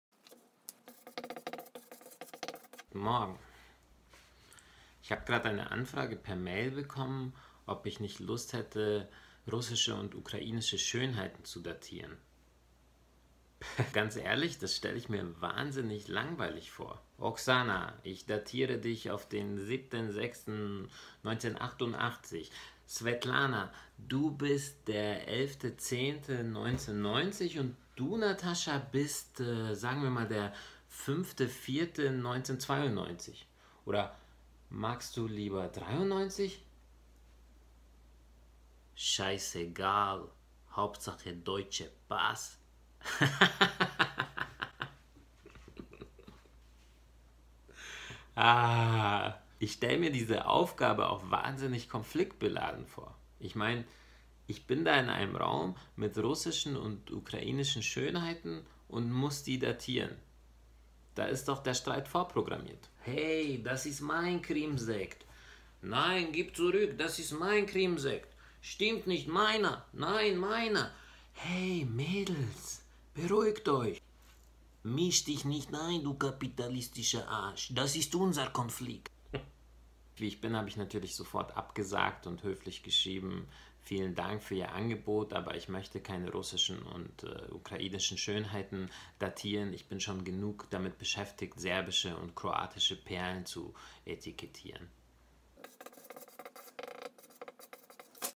Schönheiten datieren
Tag 131. Heute früh habe ich eine sehr ungewöhnliche Jobanfrage bekomme. Wie ich darauf reagiere, wird dein Denken verändern.